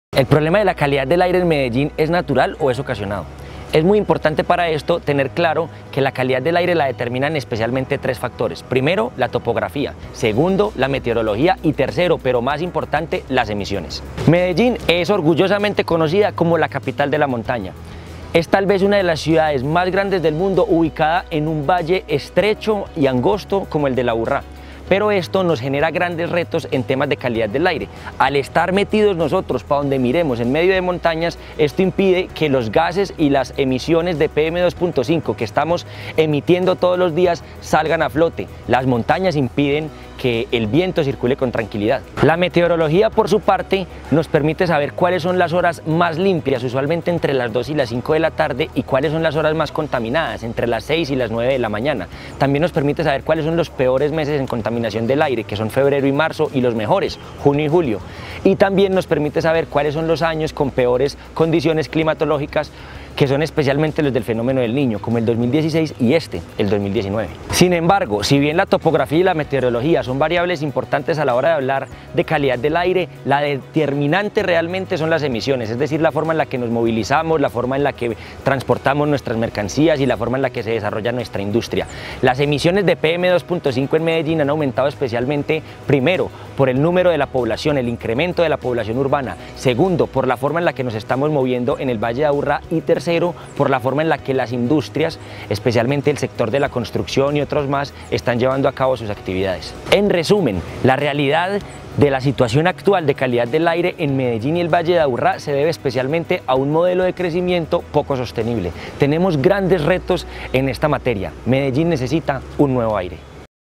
El problema de la calidad del aire ¿Es un fenómeno ocasionado o natural?
Segundo video sobre calidad del aire en Medellín